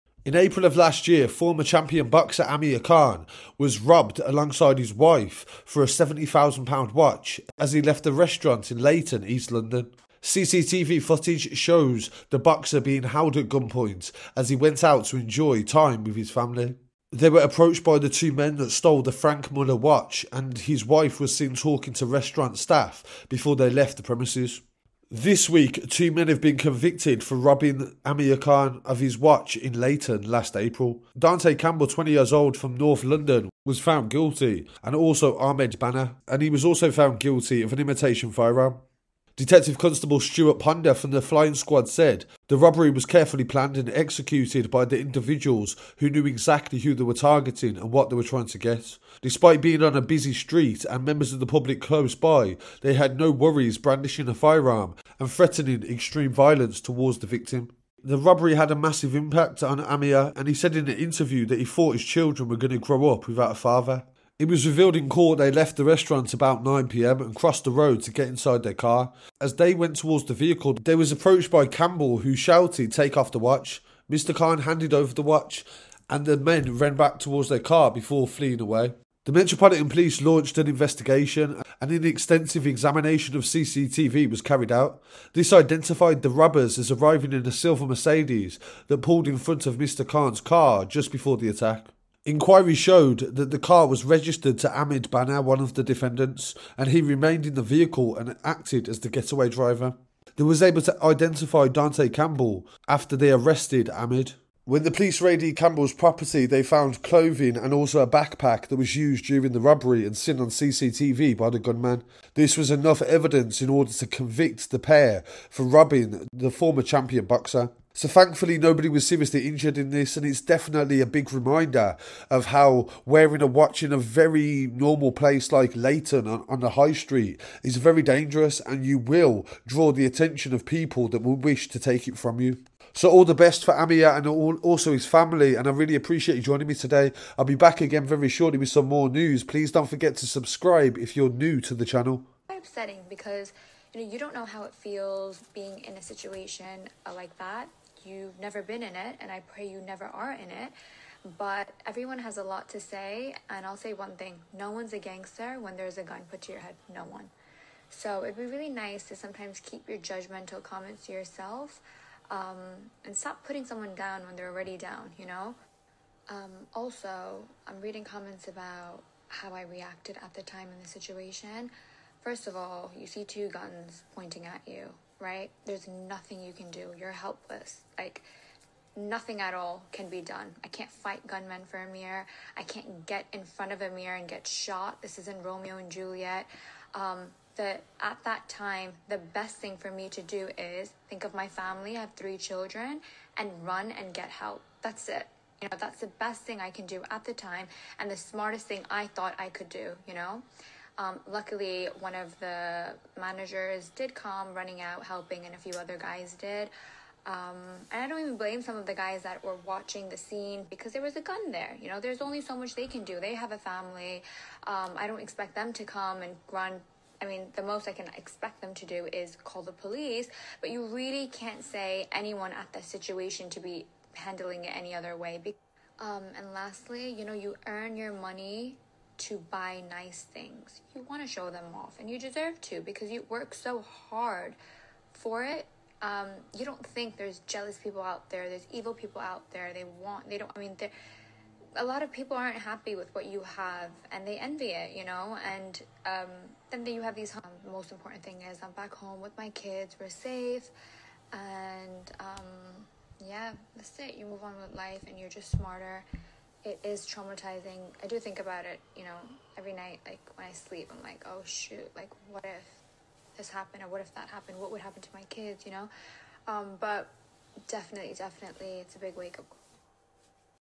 Amir Khan £70,000 Leyton gunpoint watch robbery, two men found guilty
On Monday, 18 April 2022, Mr Khan and his wife visited a restaurant on High Street, Leyton.
After eating, they left the venue at around 21:10hrs and crossed the road to get to their car. As they approached the vehicle, he was approached by Campbell, who pointed a gun in his face and shouted “take off the watch”.
Mr Khan handed over his watch – believed to be worth around £70,000 – and the men ran back towards their car before quickly driving away.